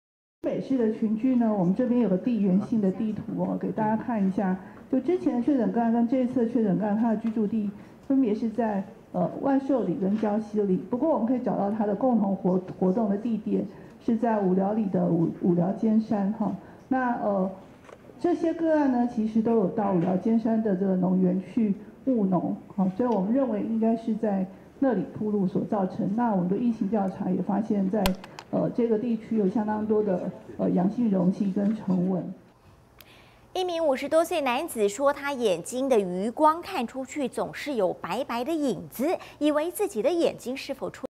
增兩例！新北爆本土登革熱
登革熱疫情持續延燒，疾管署今天公布兩例本土登革熱病例，都是住在三峽，共同地點，都有前往住家附近的菜園活動。

 疾管署防疫醫師 劉定萍：「新北市的(登革熱)群聚，我們這邊有一個地緣性的地圖，給大家看一下 之前的確診個案，跟這次的確診個案 他的居住地，分別在龍壽里跟礁溪里，不過我們可以找到，他的共同活動地點，是在五寮里的五寮尖山，這些個案都有到，五寮尖山的農園去務農，所以我們認為應該，是在那裡暴露所造成，我們的疫情調查也發現，在這個地區有相當多的，(蚊蟲)陽性容器跟成蚊。」